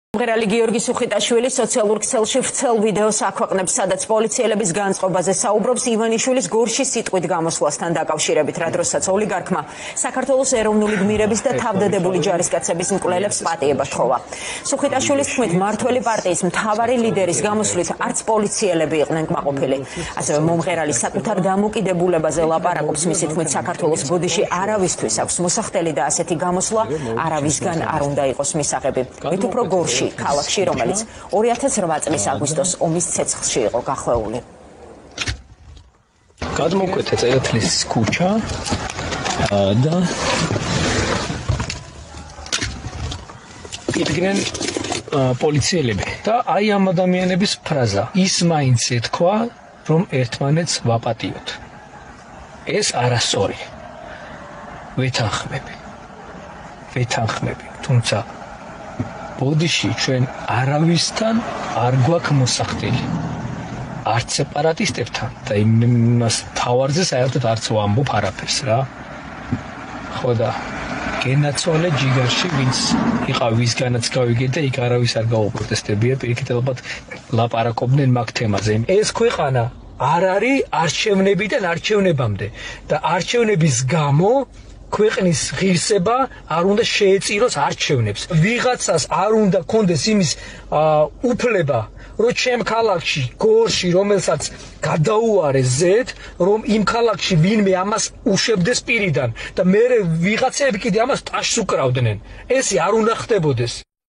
პოლიციელების განწყობები ოლიგარქის ანტისახელმწიფოებრივ განცხადებაზე
▪️ პოლიციელების განწყობები ოლიგარქის ანტისახელმწიფოებრივ განცხადებაზე | მომღერალი გიორგი სუხიტაშვილი სოციალურ ქსელში ვრცელ ვიდეოს აქვეყნებს, სადაც პოლიციელების განწყობაზე საუბროს ივანიშვილის გორში სიტყვით გამოსვლასთან დაკავშირებით